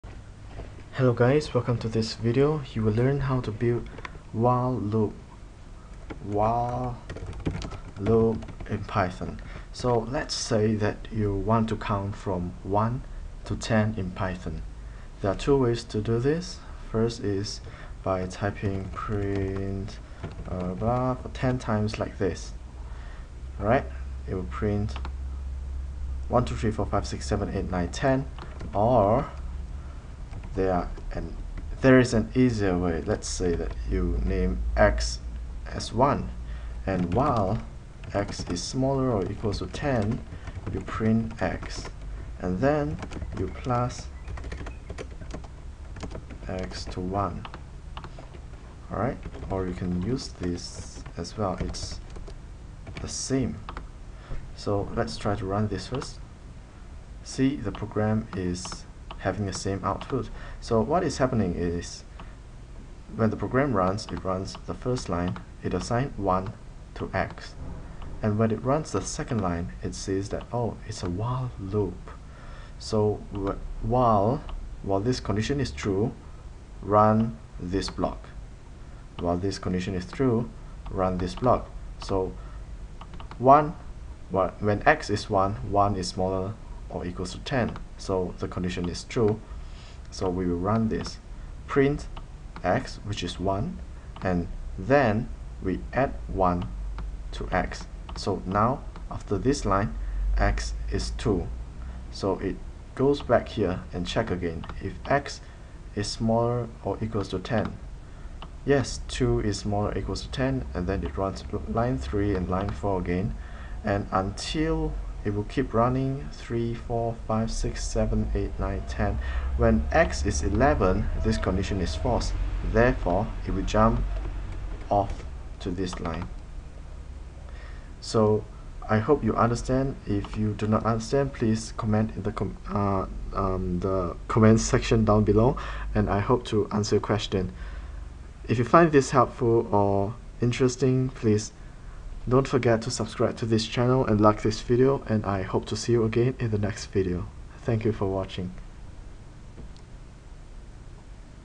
One Minute Python 007 - While Loop in Python
While loop in Python is the best way to loop through your codes if you do not know how many times you want to run the same lines of codes. If you know how many times you want to run the same lines of codes, use For loop instead.